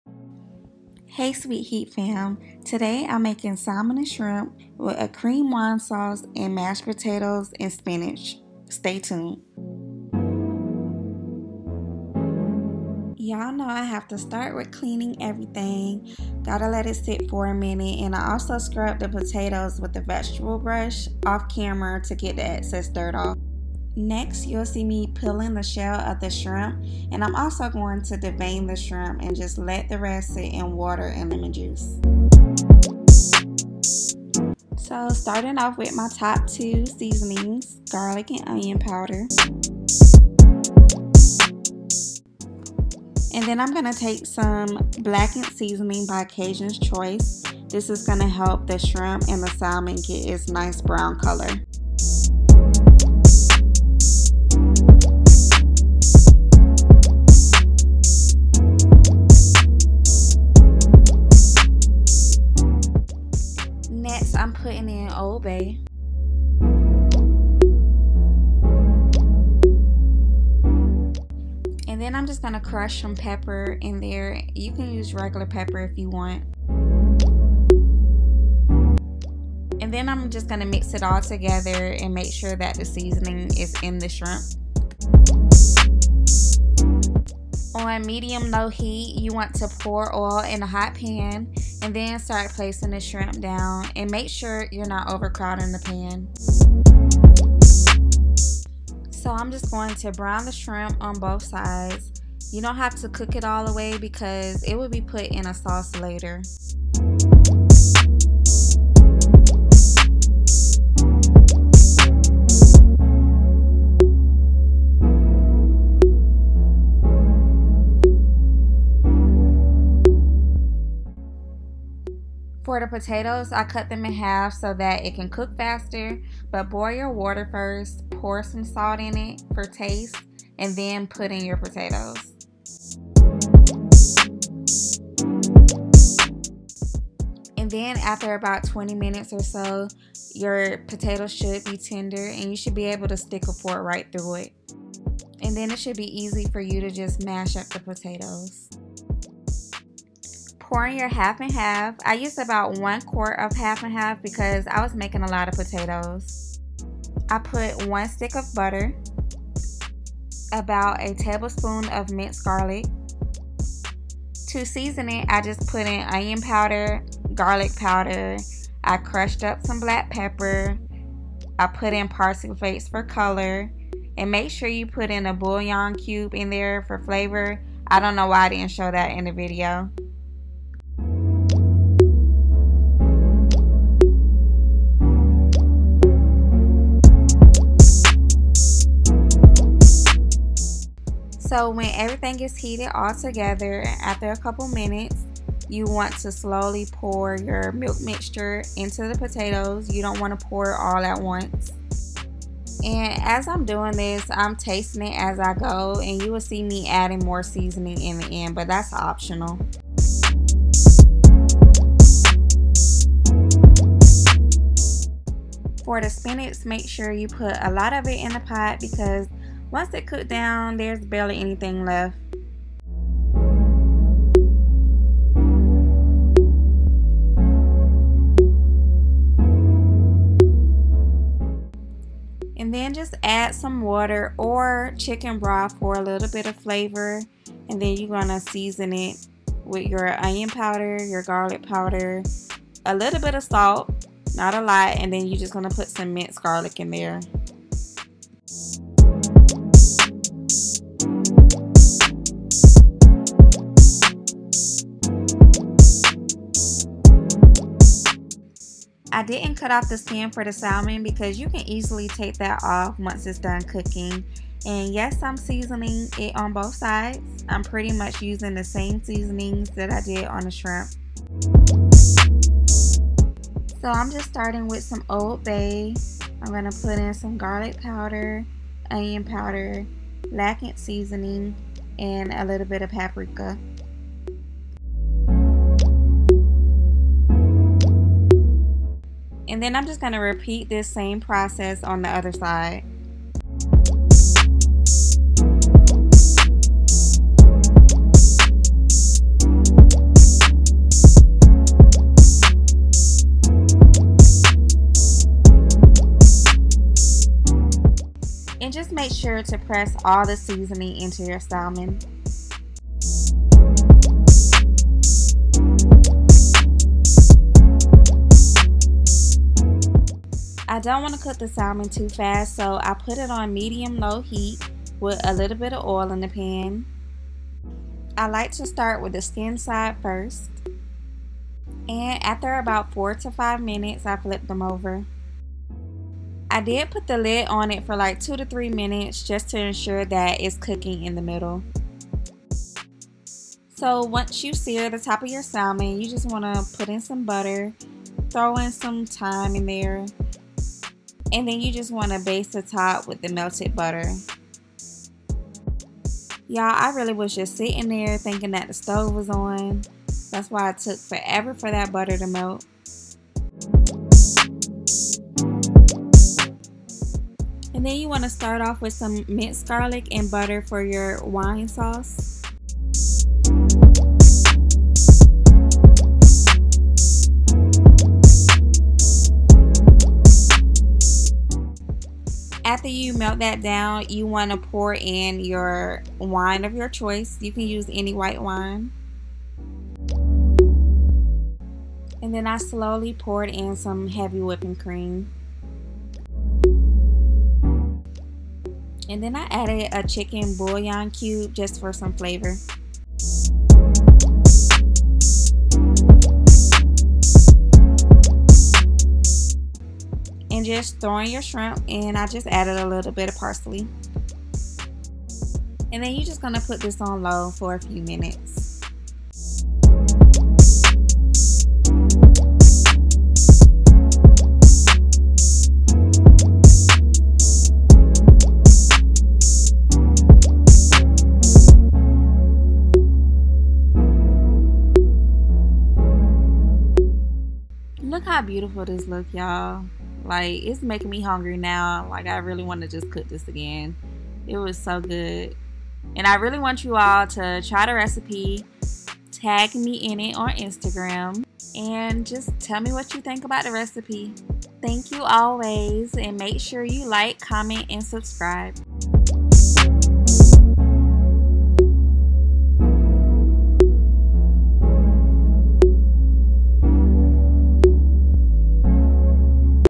The Best Salmon & Shrimp Recipe | Cream Wine Sauce | Homemade Mashed Potatoes | SweetHeatCooks
Today I made an easy recipe on how to make blackened salmon, shrimp in a garlic cream sauce, homemade mashed potatoes, & spinach

Currently at 1.7k subscribers♥️ I’m not sure why it’s not showing

Please follow and tag me on IG or Facebook to show me how this recipe turned out for you.